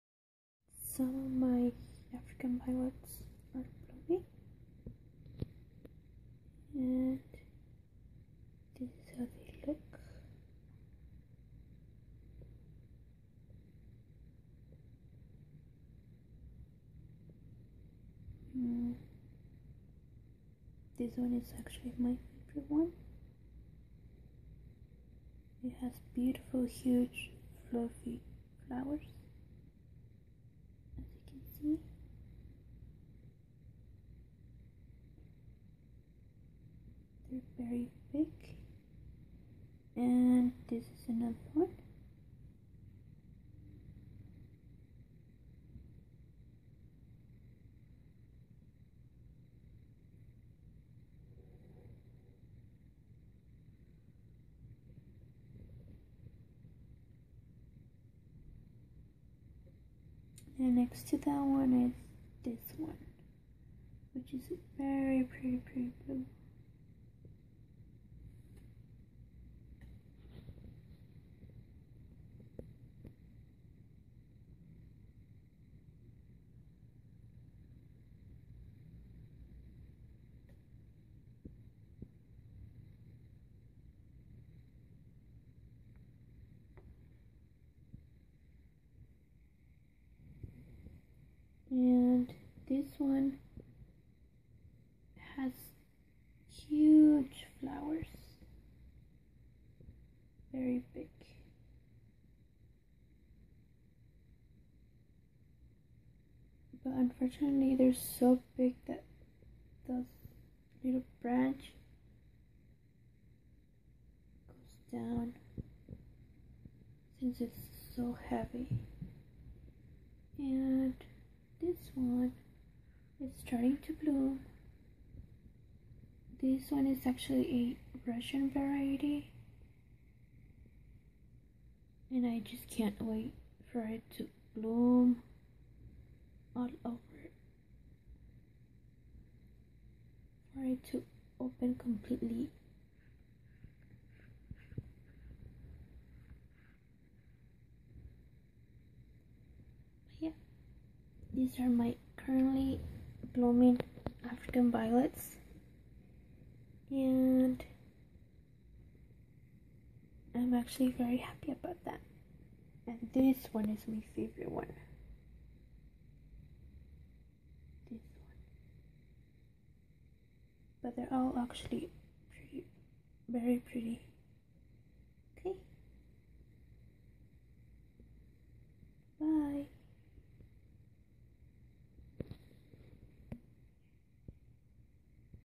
My blooming African Violets (standards)
This are my currently blooming African Violets at the moment.